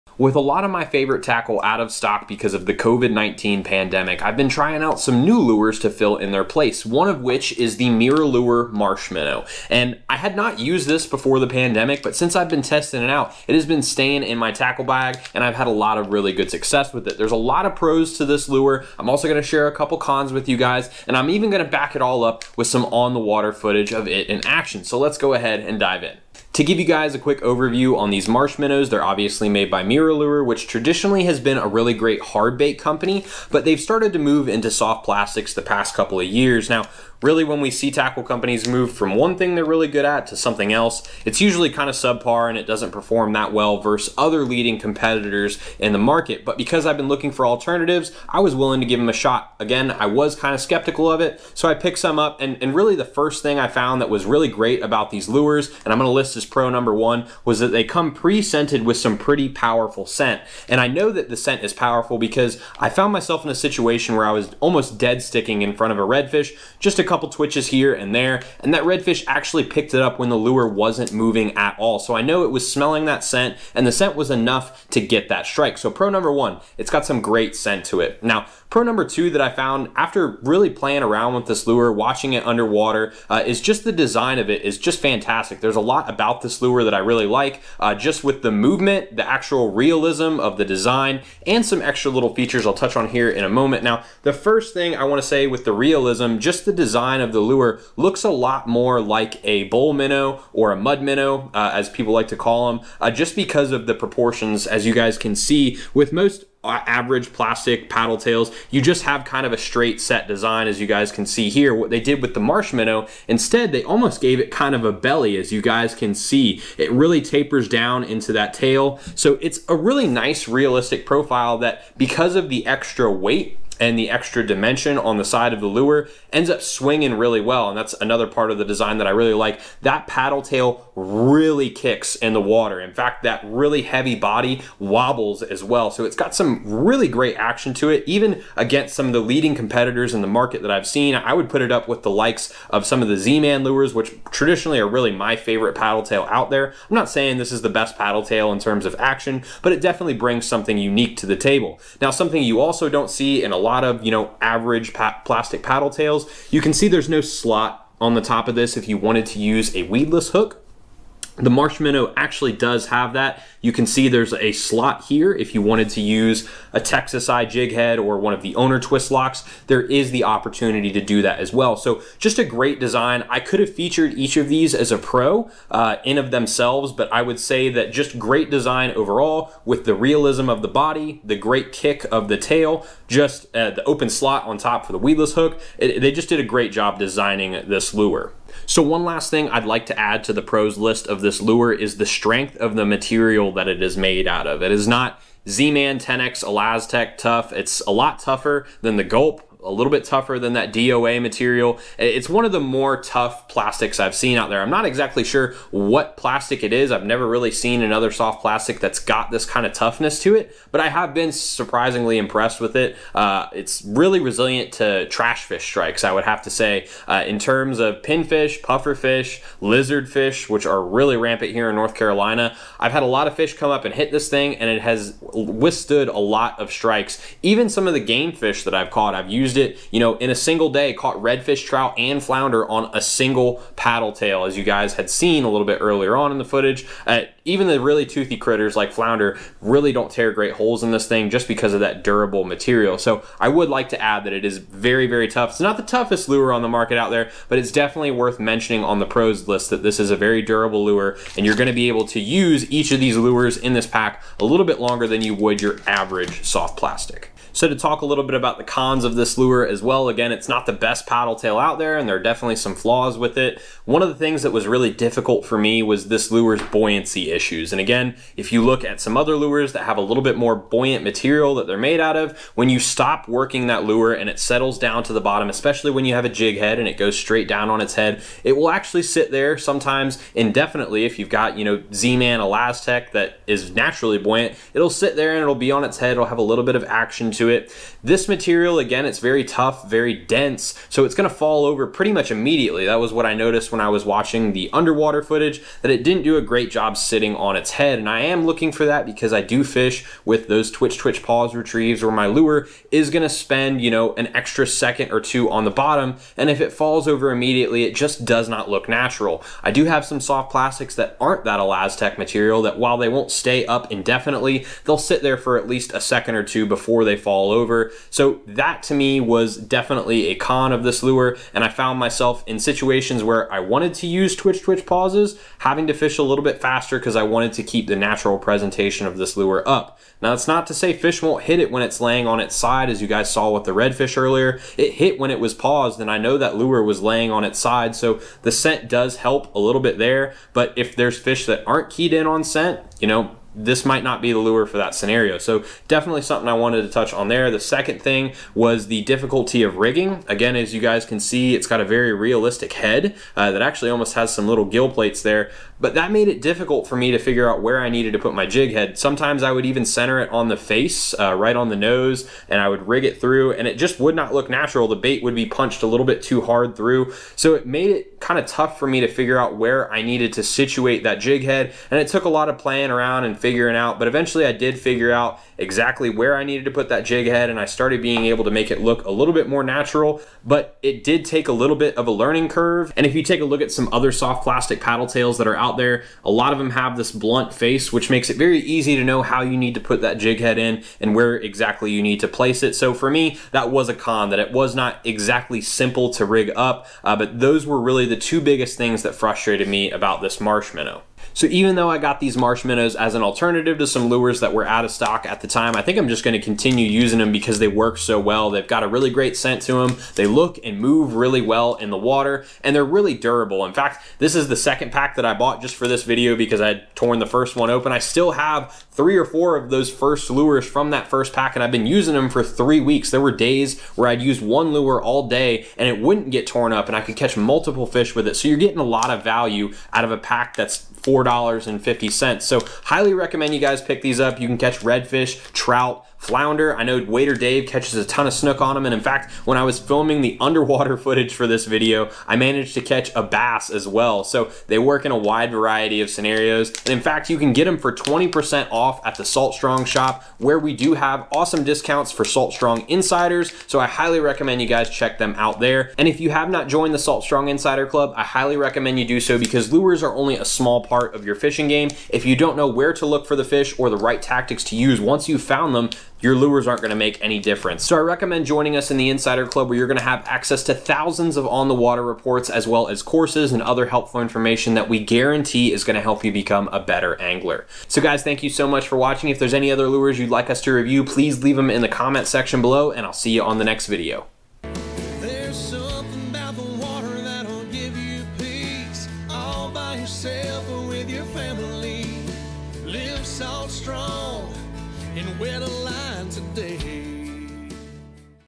MirrOlure Marsh Minnow Review (Surprisingly Good Paddletail!)
Want to stop paying full price for your tackle and get new inshore fishing spots every week?

To get the MirrOlure Marsh Minnow from our store,

It's paddletail review time!

Have you used the MirrOlure Marsh Minnow yet?

My go-to paddletails have been out of stock recently due to Covid, so I decided to try out these lures.

I was a little hesitant at first, but was pleasantly surprised at how many fish I caught with it!

In this video, I'm breaking down the top pros and cons of it, including:

- Why I was able to catch a redfish while basically deadsticking the Marsh Minnow
- Why they're great for rigging weedless
- Why you might not want to use these if you fish very slowly

Have any questions about the MirrOlure Marsh Minnow?

Have you used it before?